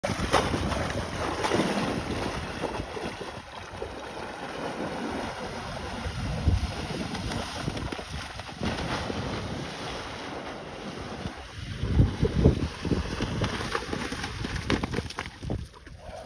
Horse Suddenly Decides He Needs a Bath || ViralHog
"21-year-old white horse was strolling around in his pond when he got the zoomies. This resulted in him taking a big dive into the water!"

Occurred on March 29, 2024 / Breda, Noord Brabant, Netherlands